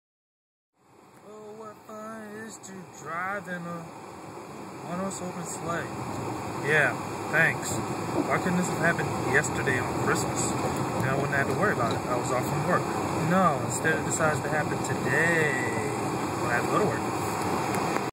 Homeward Snow Bound
Driving home in the snow storm. 12/26/12